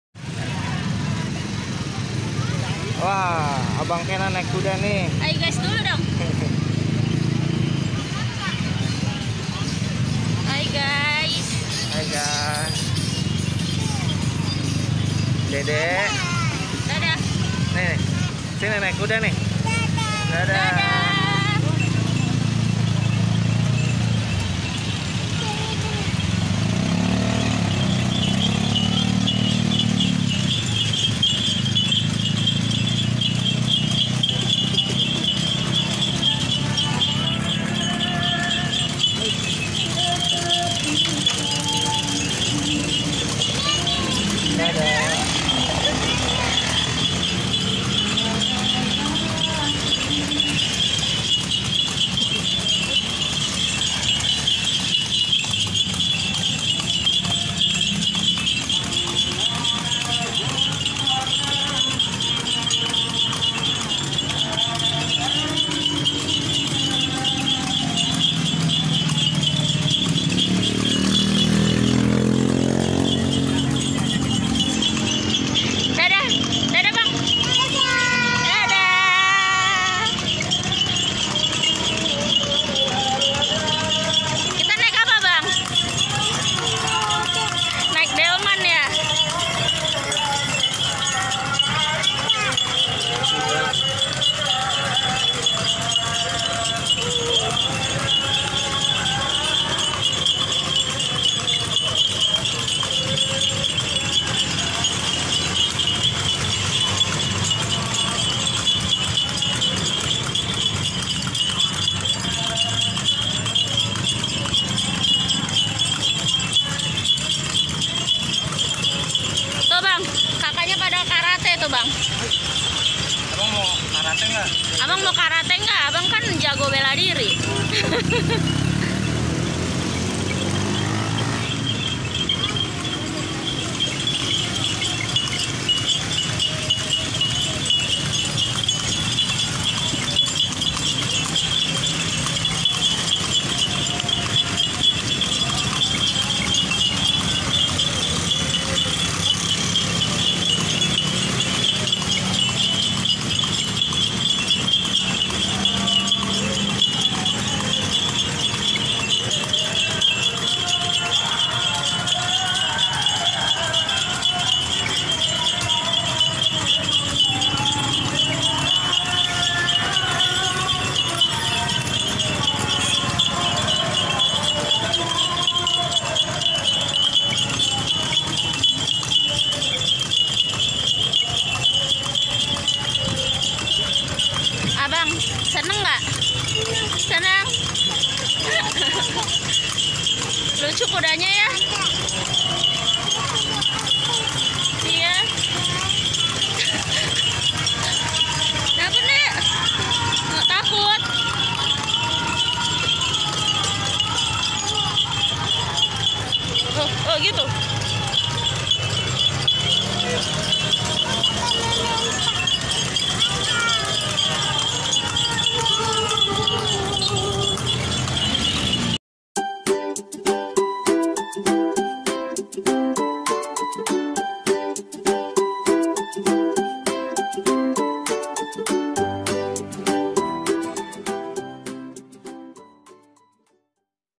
Delman Sultan!! Keenan Keanu Naik Delman di Jakarta Islamic Centre
Keenan Keanu Naik Delman di Jakarta Islamic Center